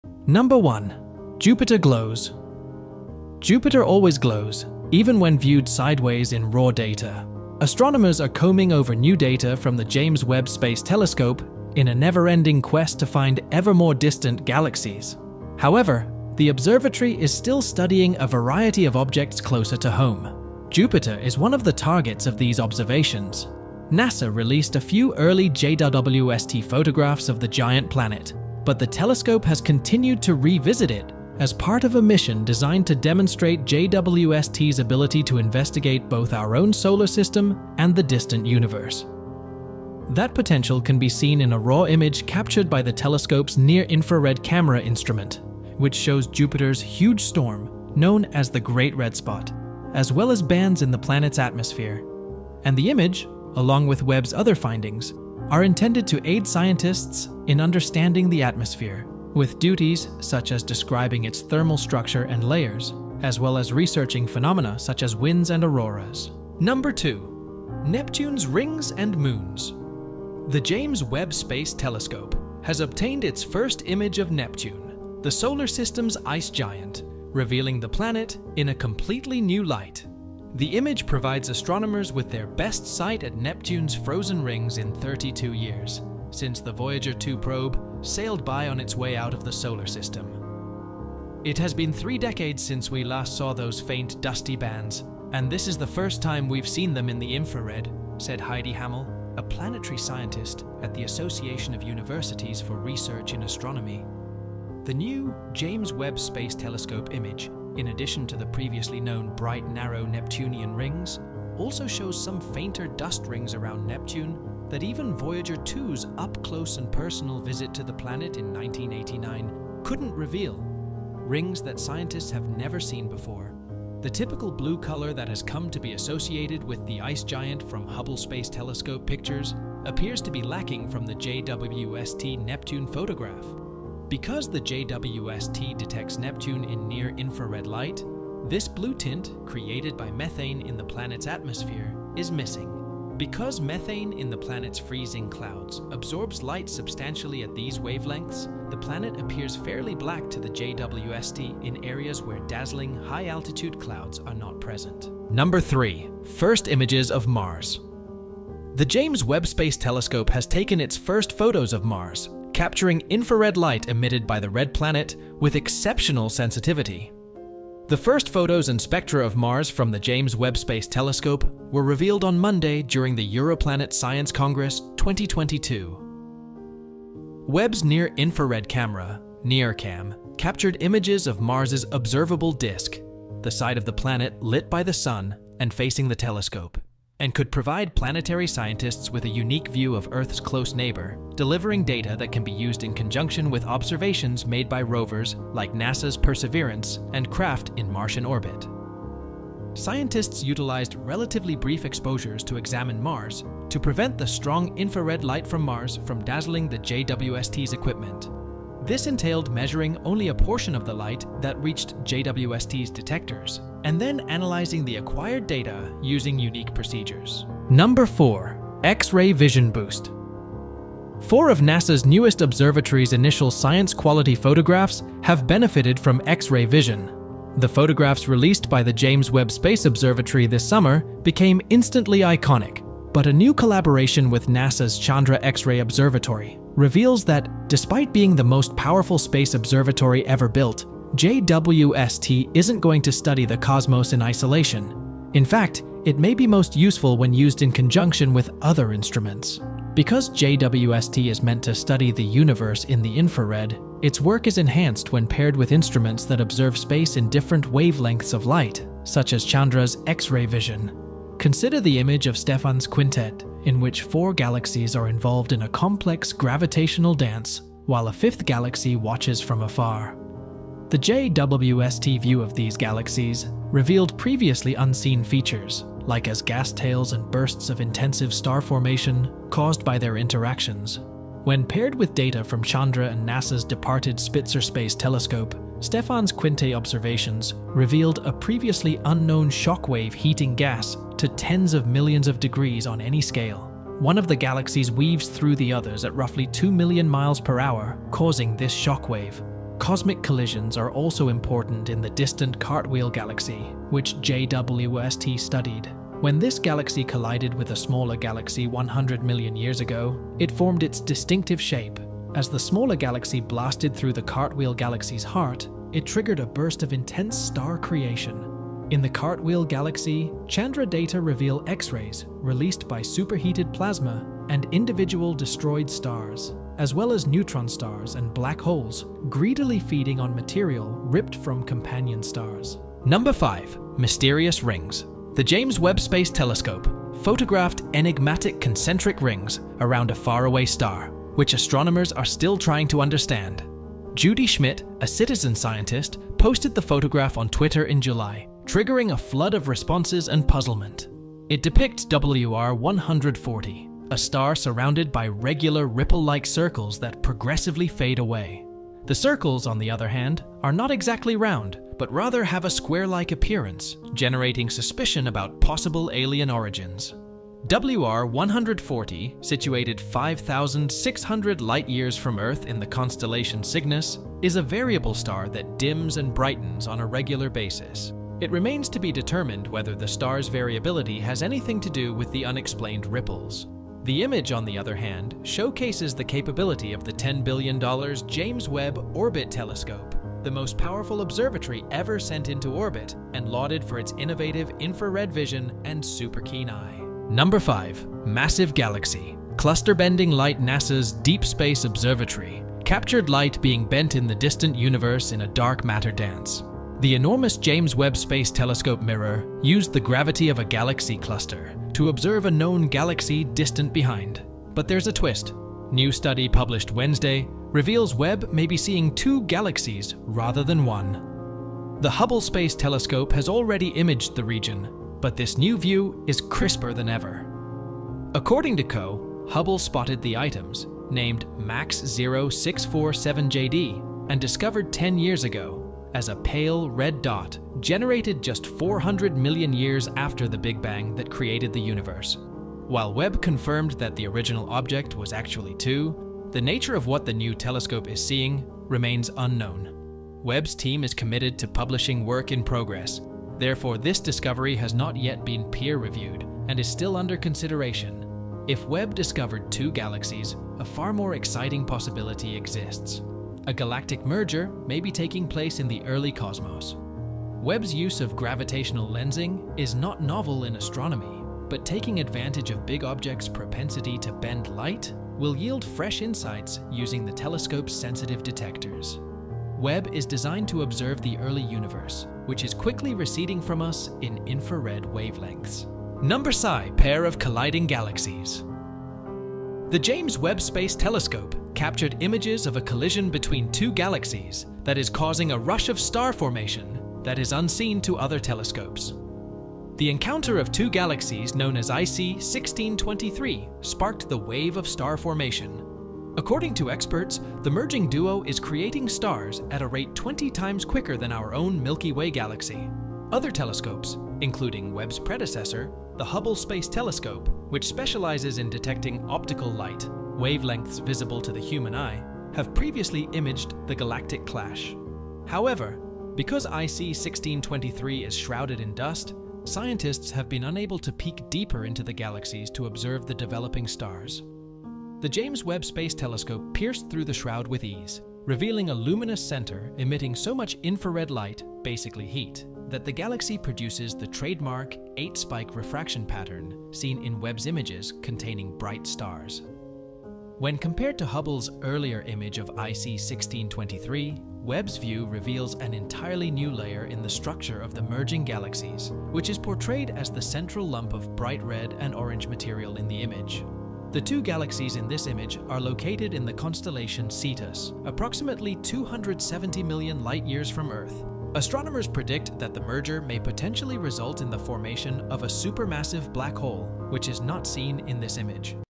James Webb Space Telescope 7 NEW, Real Images From Outer Space
00:00 Jupiter glows
01:06 Neptune's rings and moons
02:35 1st images of Mars
03:37 X-ray vision boost
05:32 Mysterious rings
06:37 Massive galaxy cluster bending light
08:06 pair of colliding galaxies